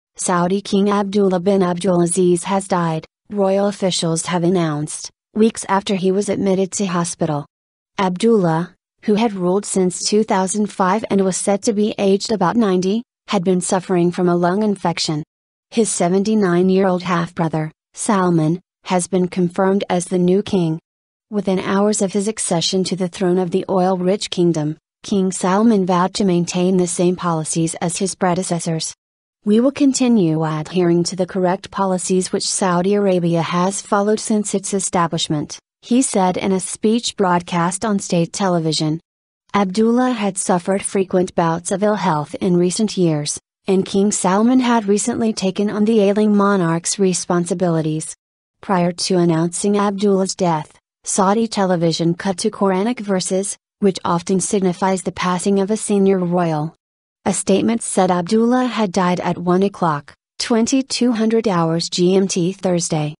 Saudi Arabia's King Abdullah Dies: Breaking News
Saudi King Abdullah bin Abdulaziz has died, royal officials have announced, weeks after he was admitted to hospital.

Abdullah, who had ruled since 2005 and was said to be aged about 90, had been suffering from a lung infection.

His 79-year-old half-brother, Salman, has been confirmed as the new king.

Within hours of his accession to the throne of the oil-rich kingdom, King Salman vowed to maintain the same policies as his predecessors.

"We will continue adhering to the correct policies which Saudi Arabia has followed since its establishment," he said in a speech broadcast on state television.

Abdullah had suffered frequent bouts of ill health in recent years, and King Salman had recently taken on the ailing monarch's responsibilities.

Prior to announcing Abdullah's death, Saudi television cut to Koranic verses, which often signifies the passing of a senior royal.

A statement said Abdullah had died at 01:00 (22:00 GMT Thursday).